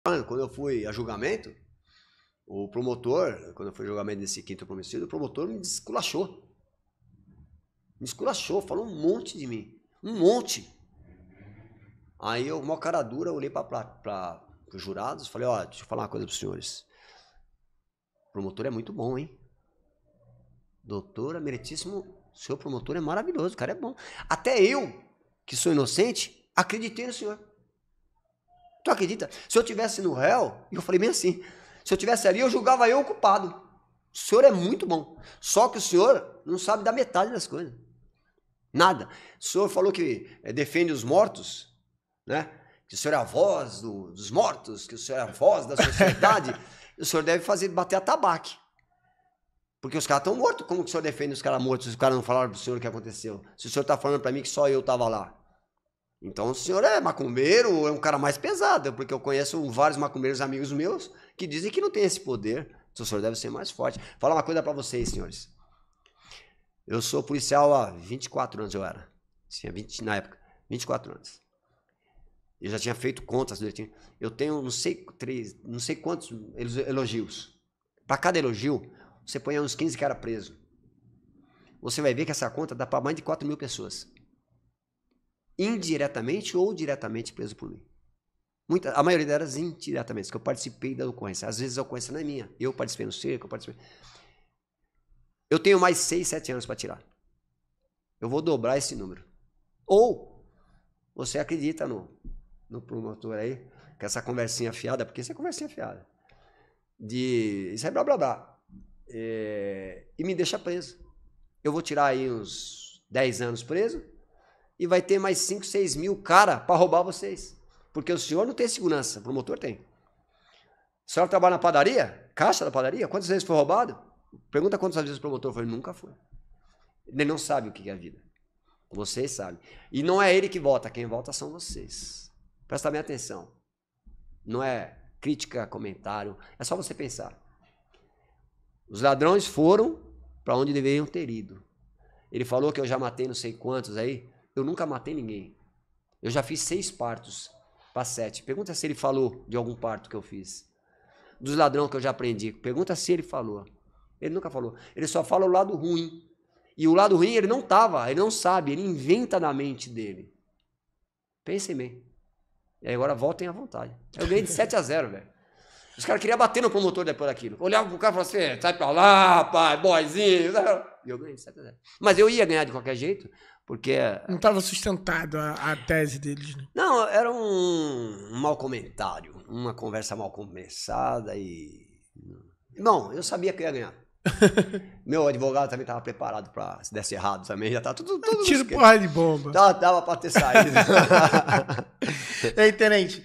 PROMOTOR ME TRATOU IGUAL BANDIDO NO JÚRI POPULAR | MAURICIO BRITES |COP CAST
G1- 🚔 PATAMEIROS
G2- 🇪🇸 TROPA DO BARÇA